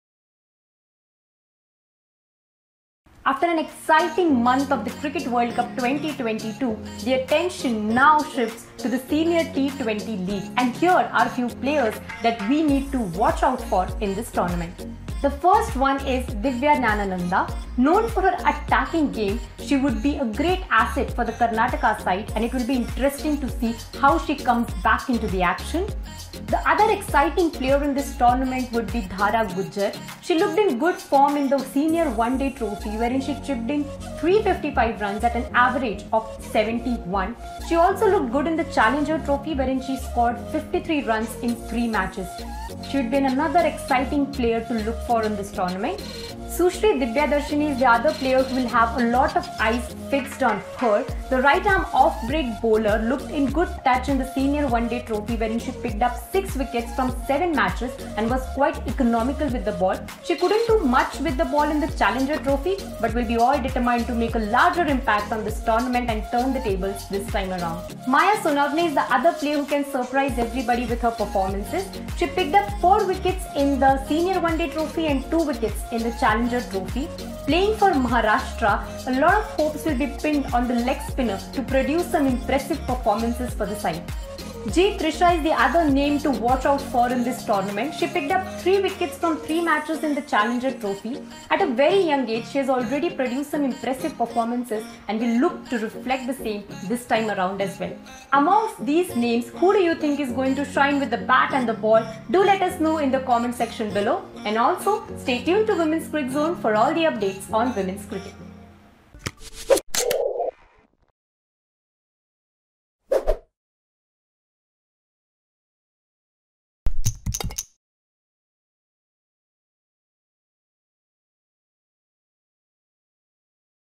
Senior T20 league - Young players to watch out for
The Senior T20 league has kicked off already, but who are the young players to watch out for in this exciting T20 battle.